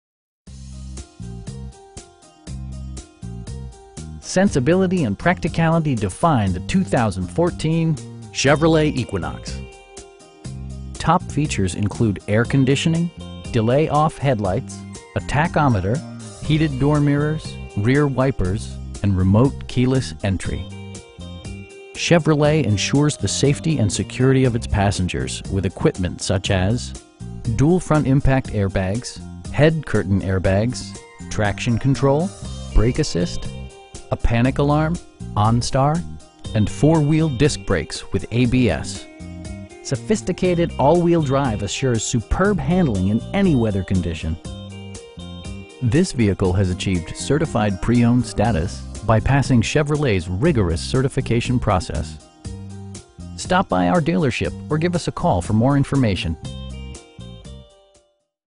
2014 Chevrolet Equinox 1LT AWD Auto w/ Heated Seats
2595 Barnett Highway.

Sensibility and practicality define the 2014 Chevrolet Equinox. Top features include air conditioning, delay-off headlights, a tachometer, front bucket seats, tilt and telescoping steering wheel, heated door mirrors, rear wipers, and remote keyless entry. Chevrolet ensures the safety and security of its passengers with equipment such as: dual front impact airbags, head curtain airbags, traction control, brake assist, a panic alarm, onStar, and 4 wheel disc brakes with ABS. Sophisticated all wheel drive assures superb handling in any weather condition. This vehicle has achieved Certified Pre-Owned status, by passing Chevrolet's rigorous certification process. We pride ourselves on providing excellent customer service. Stop by our dealership or give us a call for more information.